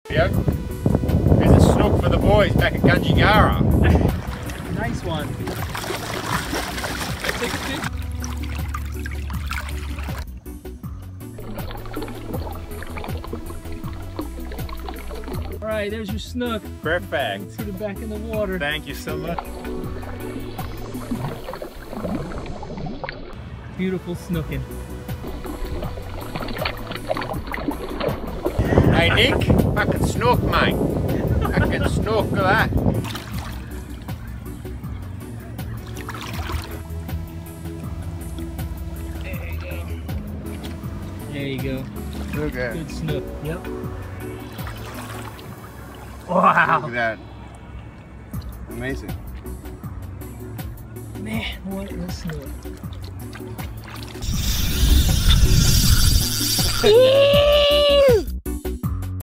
Miami Snook Fishing Highlights
Some highlights from the last few charters snook fishing in Miami

SOUNDTRACK